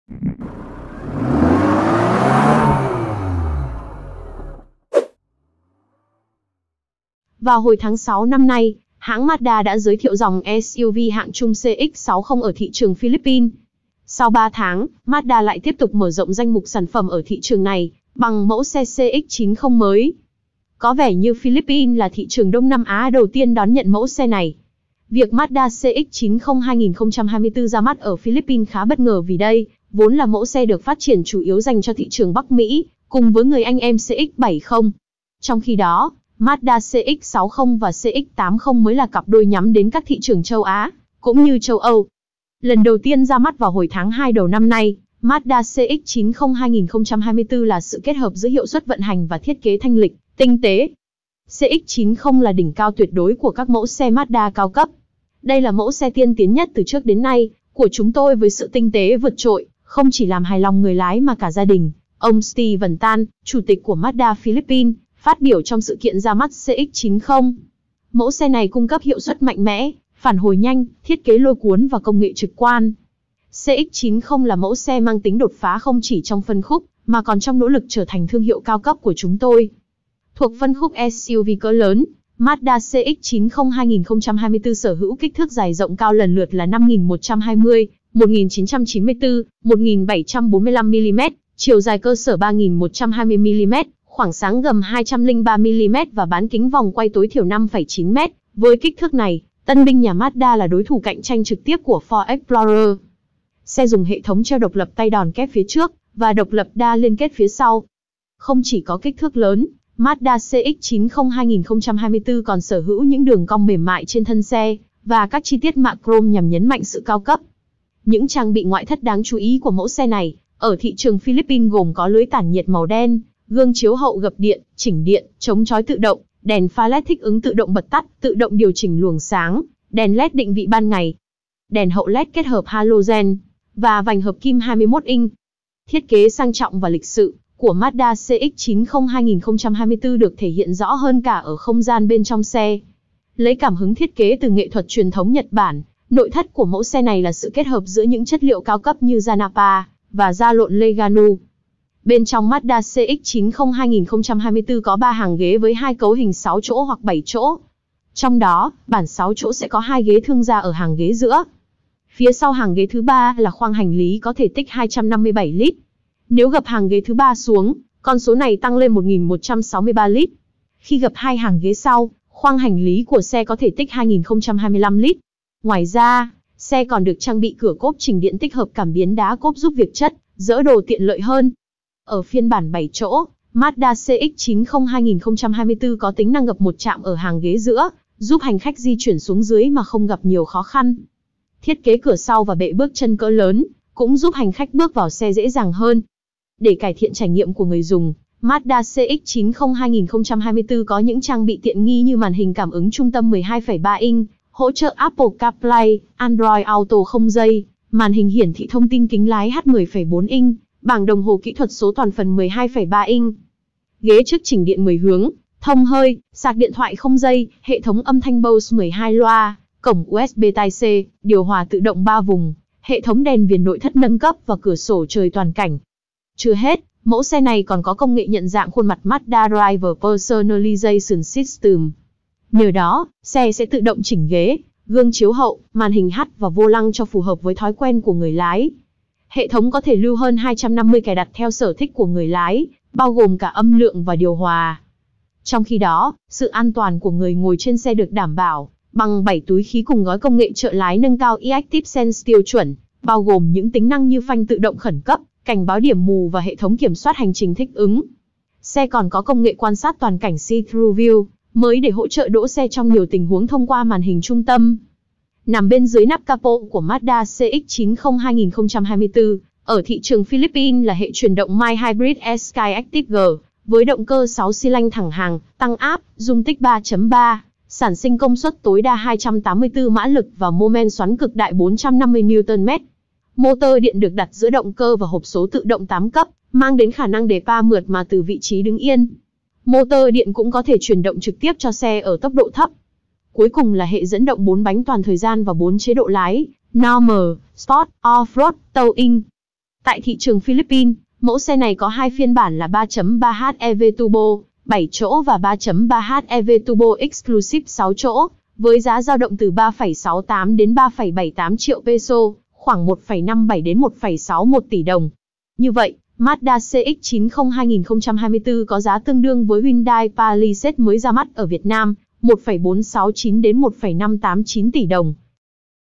Sau CX-60, đến lượt Mazda CX-90 2024 cập bến Đông Nam Á, giá tương đương Hyundai Palisade ở Việt Nam
360 Xe_Việc Mazda CX-90 2024 ra mắt ở thị trường Philippines khá bất ngờ vì đây vốn là mẫu xe được phát triển chủ yếu dành cho thị trường Bắc Mỹ.